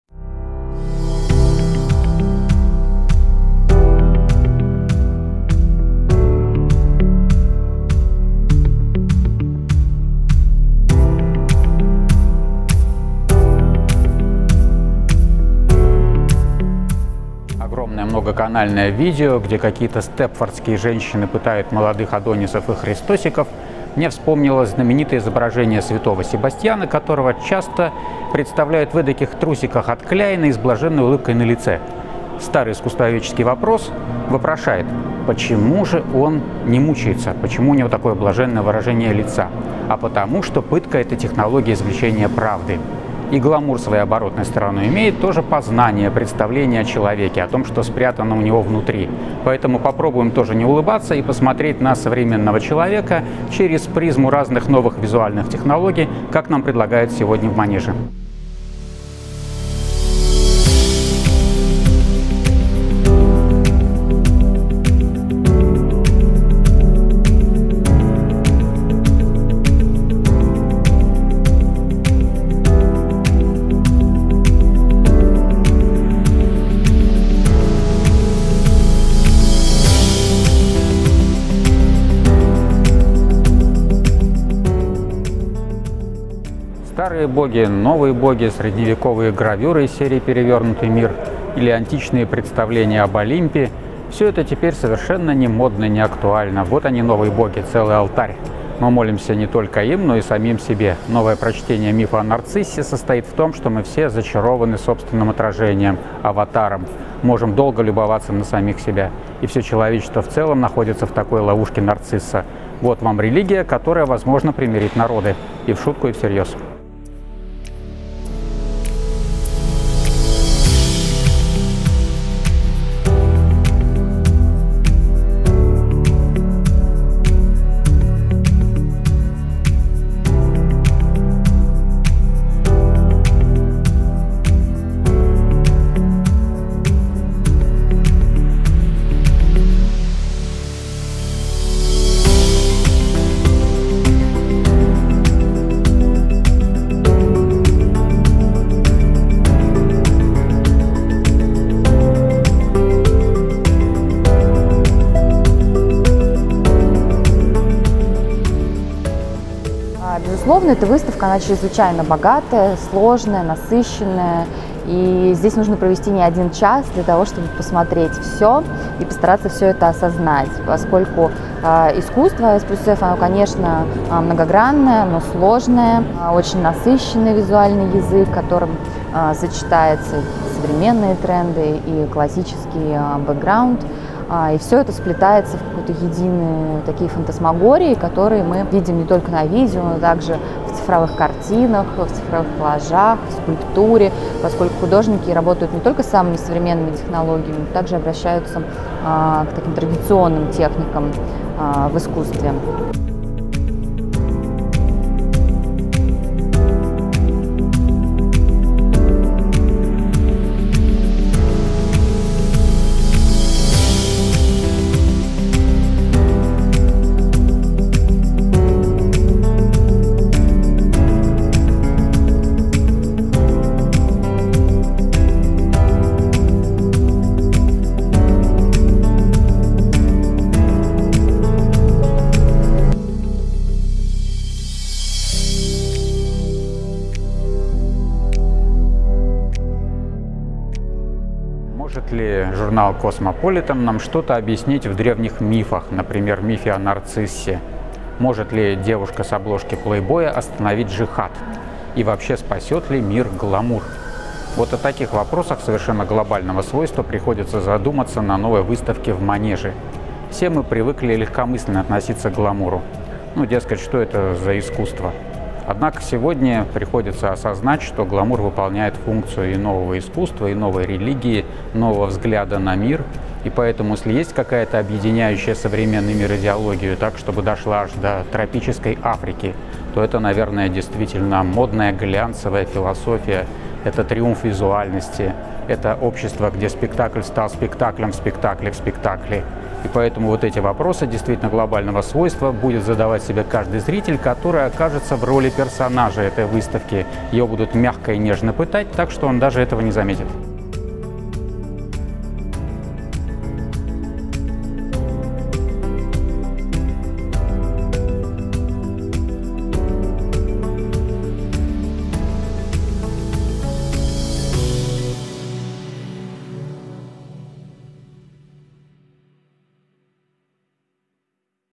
AES+F. ПРЕДСКАЗАНИЯ И ОТКРОВЕНИЯ : выставка в Манеже АРТЛИКБЕЗ № 242
На известной картине Антонелло да Мессина прославленный христианский мученик св. Себастьян изображен приятным молодым человеком в модных трусиках. Да, он пронизан стрелами и привязан к дереву, но судя по выражению лица и позе, Себастьян не слишком мучается, даже получает удовольствие. Наивный вопрос "почему мука так приятна?" имеет одно решение: дело в том, что испокон веков пытка рассматривалась как технология познания, извлечения правды. Палачи и жертвы выступали в ней сотрудничающими сторонами, а цель - обретение истины - оправдывала и возвышала мелкие страдания и неудобства.
На выставке моднейших и раскрученных художников объединения AES+F в Манеже вспоминаются и эти гламурные пытки и брендовые трусики Себастьяна - ведь в грандиозных видеоинсталляциях здесь тоже нежно пытают юных адонисов и христосиков. Стилизованные степфордские женщины мягко обрабатывают своих визави на дыбах производства ИКЕА, защелкивают хромированные колодки, вращают пыточные барабаны с жертвами прохладной гендерной войны... Делается это тоже для удовольствия, но и для совместного поиска истины. Той истины, что именно гламур становится сегодня ведущей мировой религией, главной дорогой самопознания и самореализации. Все мы нарциссически зачарованы собственными аватарами, прикованы к аккаунту в инстаграме, добровольно-принудительно кооптированы в бесконечный спектакль. Нам неудобно, но терпимо, слегка противно от переизбытка глянца, но без косметики и фотошопа - все равно никуда...
Так примирит ли нас, все возрасты и расы эта невыносимая красота? Снизойдут ли в благословенных лайках и перепостах на наши страницы боги гламура и иконы стиля? Может ли остановить джихад девушка с обложки "Плейбоя"? Вот какие вопросы задает себе зритель выставки "Предсказания и откровения", которого тоже толерантно пытают - высокой четкостью изображения и не влезающим в обычный угол зрения сверхшироким экраном. Но тот, кто выдержит пытку до конца - придет к истинному озарению.

АРТЛИКБЕЗ № 242
арт-гид: Вячеслав Корнев
оператор и монтажер: Гера Героев
24 июля 2019, Санкт-Петербург